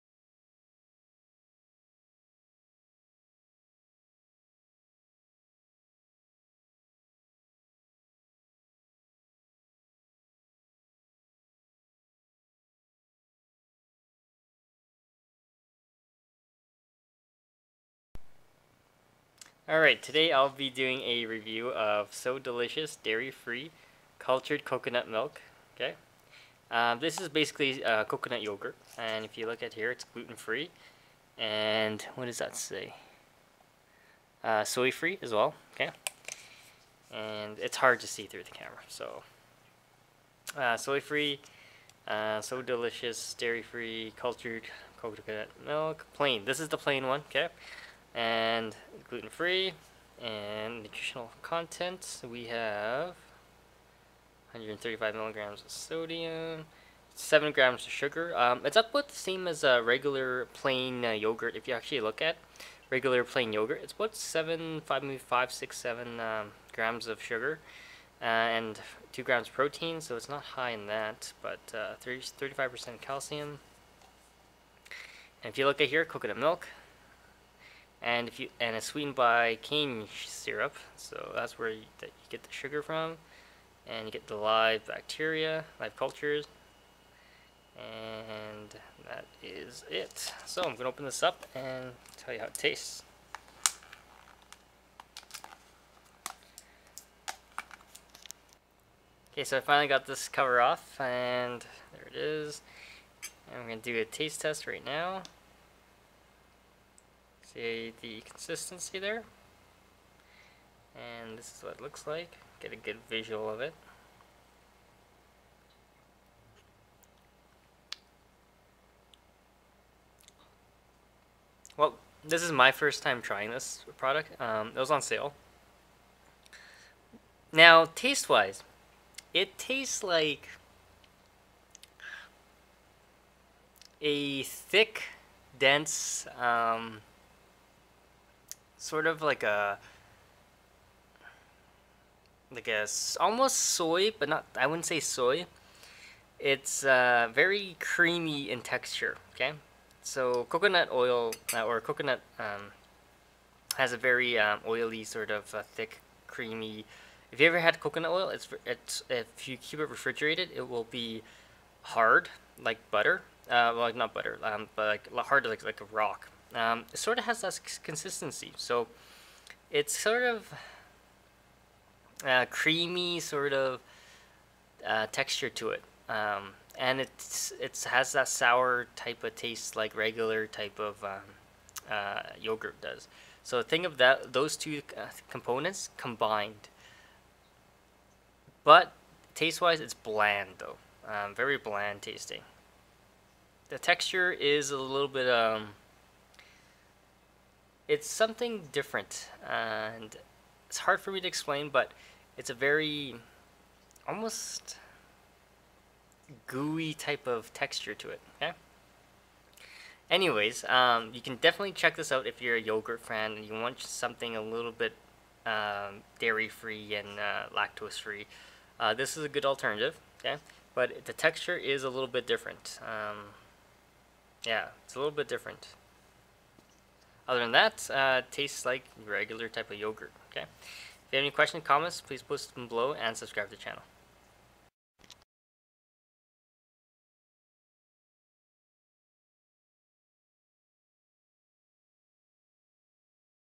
Review So Delicious Coconut Yogurt Cultured Milk Plain Dairy casein gluten free dairy alternative
Review So Delicious Coconut Yogurt Cultured Milk Plain. The coconut Yogurt is Vegan and Completely Dairy free including the live cultures.